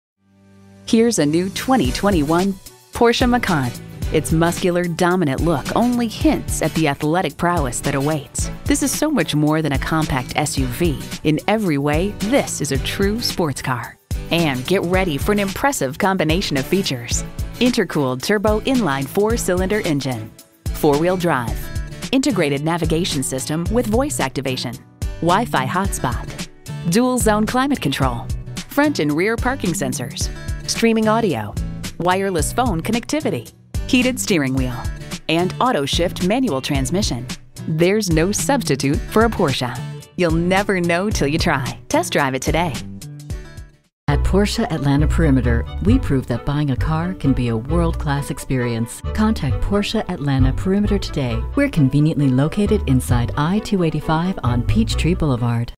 New 2021 Porsche Macan Atlanta, GA P17664 - SOLD
Year: 2021
Make: Porsche
Model: Macan
Trim: Base
Engine: 2.0L I4 Turbocharged
Transmission: 7-Speed Porsche Doppelkupplung (PDK)
Color: Blue
Interior: beige
Stock #: P17664
VIN: WP1AA2A56MLB10805

Address:
4006 Carver Drive